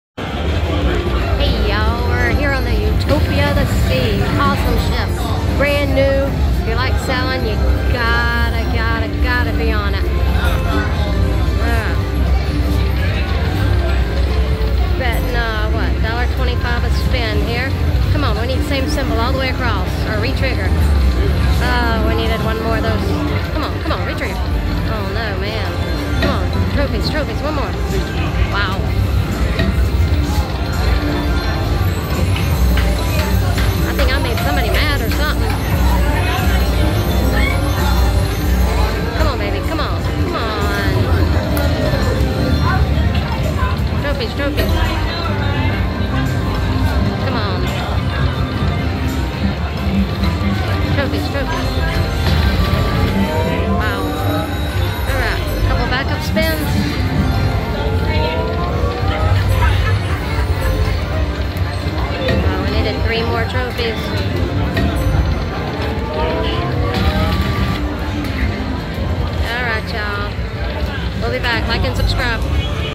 Horse Race 🏇 Bonus on Lightning $1.25 bet
On Utopia of the Seas playing this Horse Race game